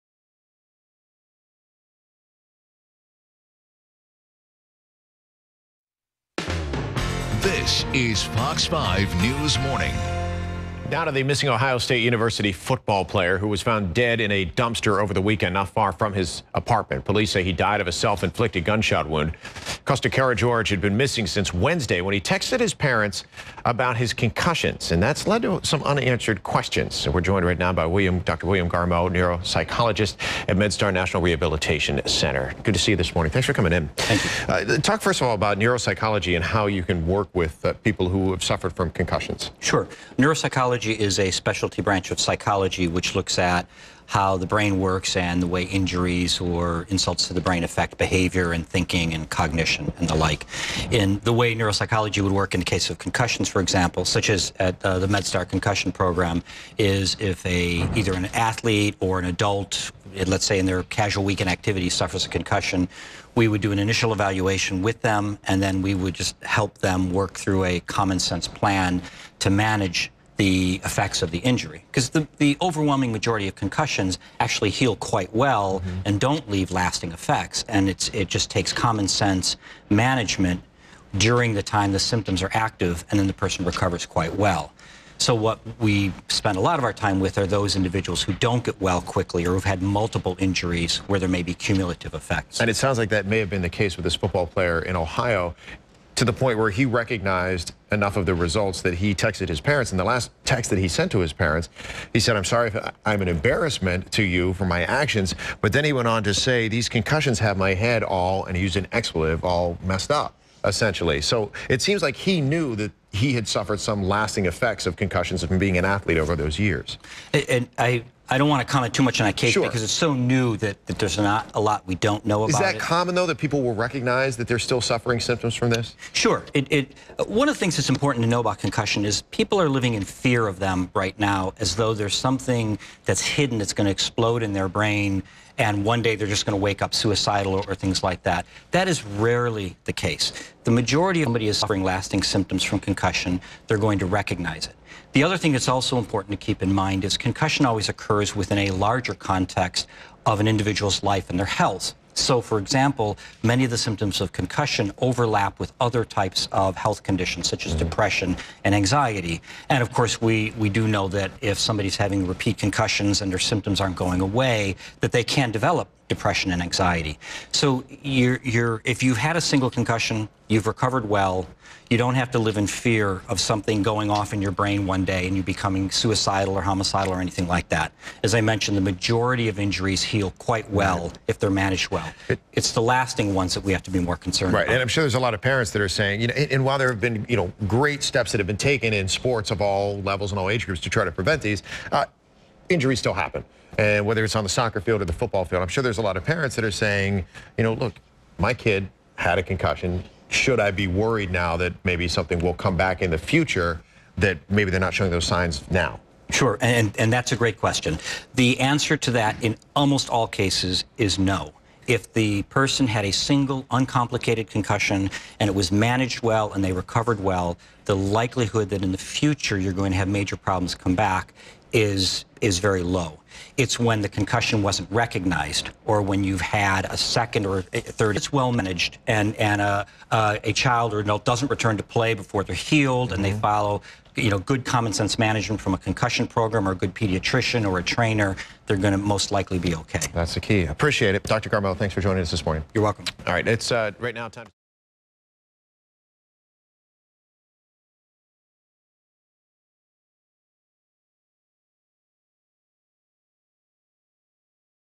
WTTG-FOX: Dr. William Garmoe - Neuropsychology and Concussions
William Garmoe, PHD, with the MedStar National Rehabilitation Network speaks with WTTG's Steve Chenevey about Neuropsychology and Concussions Management. and Concussions Management services